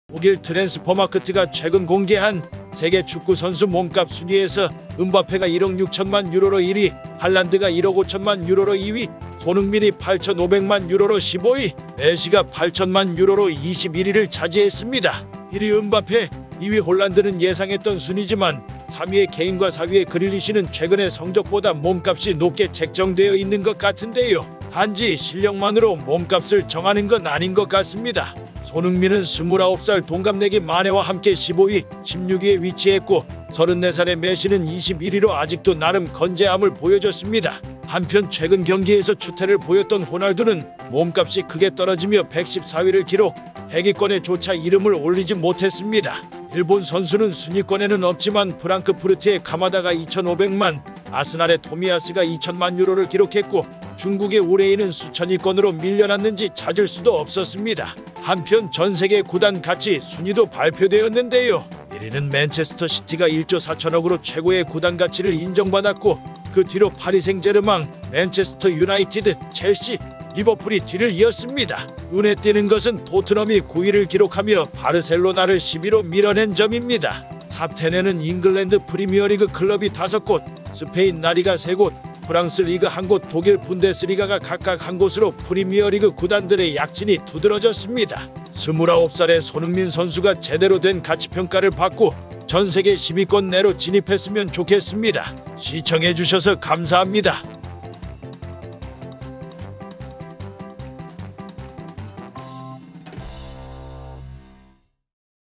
세계 축구선수 몸값 순위!!! 손흥민 1163억 몇위??
독일 transfermarkt이 최근 공개한 세계 축구 선수 몸값 순위에서 음바페가 1억6000만 유로로 1위, 할란드가 1억5000만 유로로 2위, 손흥민이 8500만 유로(1163억)로 15위, 메시가  8000만 유로(1094억)로 21위를 차지했습니다.

1위 음바페, 2위 홀란드는 예상했던 순위지만 3위의 케인(1642억)과 4위의 그릴리쉬는 최근의 성적보다 몸값이 높게 책정되어 있는 것 같은데요. 단지 실력만으로 몸값을 정하는 건 아닌 것 같습니다.

손흥민은 29살 동갑내기 마네와 함께 15,16위에 위치했고 34살의 메시는 21위로 아직도 나름 건재함을 보여줬습니다.

한편 최근 경기에서 추태를 보였던 호날두는 몸값이 크게 떨어지며 114위를 기록, 100위권에 조차 이름을 올리지 못했습니다.
일본 선수는 순위권에는 없지만 프랑크 프르트의 카마다가 2500만, 아스날의 토미야스가 2000만유로를 기록했고 중국의 우레이는 수천위권으로 밀려났는지 찾을 수도 없었습니다.
한편 전세계 구단 가치 순위도 발표되었는데요.
1위는 맨체스터 시티가 1조 4000억으로 최고의 구단가치를 인정받았고 그 뒤로 파리생제르망, 맨체스터 유나이티드, 첼시, 리버풀이 뒤를 이었습니다. 눈에 띄는 것은 토트넘이 9위를 기록하며 바르셀로나를 10위로 밀어낸 점입니다.
TOP 10에는 잉글랜드 프리미어리그 클럽이 5곳, 스페인 라 리가 3곳, 프랑스 리그1곳, 독일 분데스리가가 각각 1곳으로 프리미어리그 구단들의 약진이 두들어졌습니다.

29살의 손흥민 선수가 제대로 된 가치평가를 받고 전세계 10위권 내로 진입했으면 좋겠습니다.